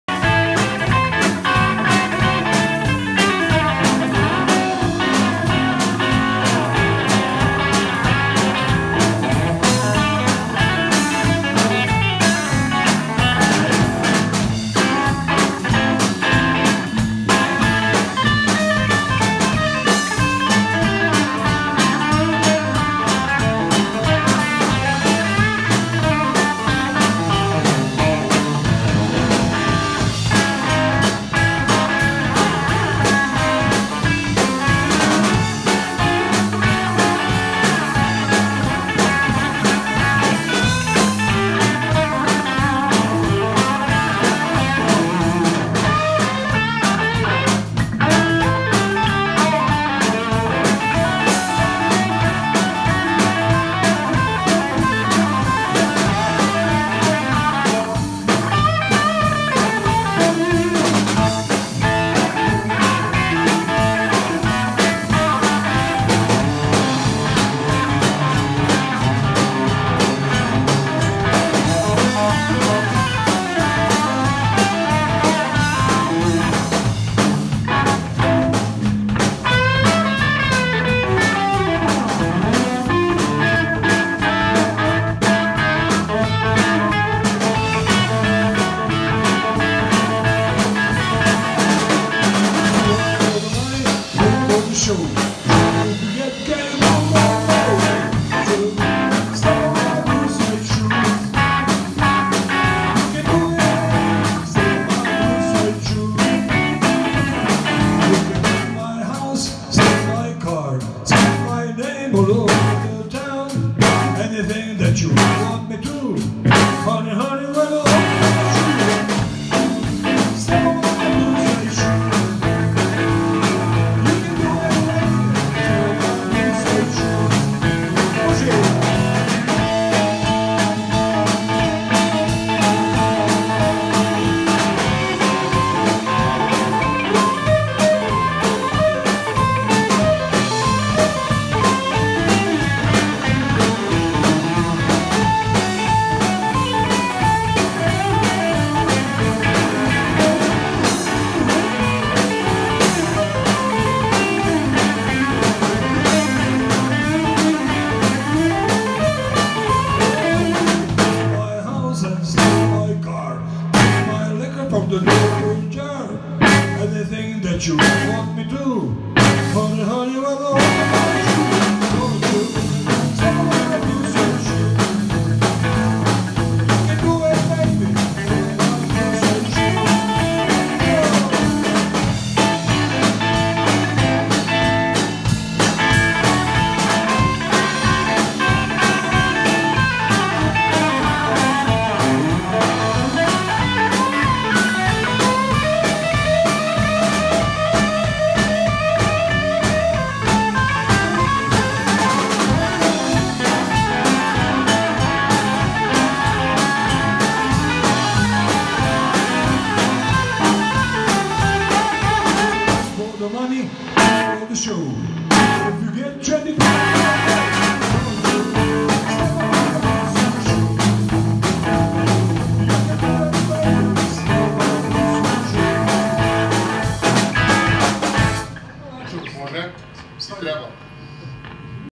Borislav Mitic lead guitar solo - Blues Rock style. Soundcheck
Soundcheck lead guitar improvisation, Rockabilly style in a small club in Belgrade with Vlada Dzet Band.